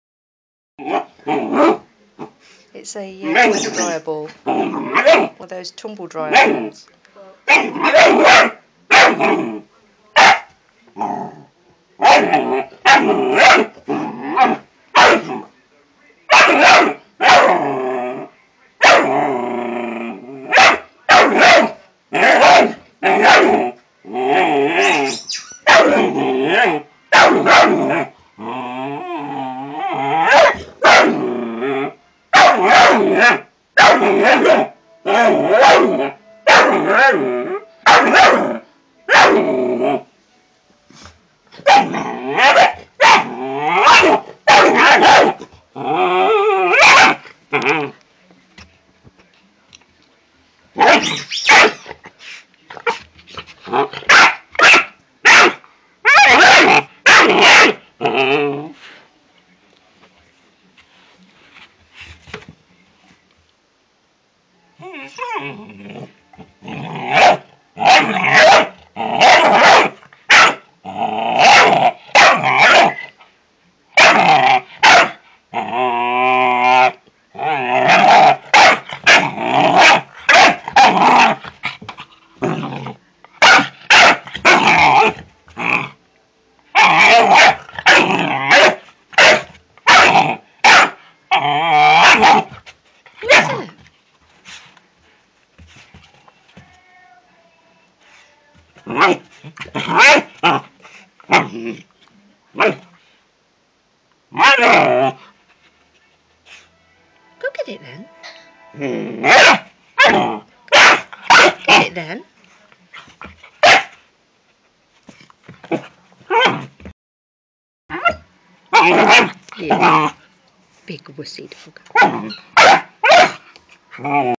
Tia the Wussy dog.
My 3 month old Doberman, protecting me from a tumble dryer ball! This video was uploaded from an Android phone.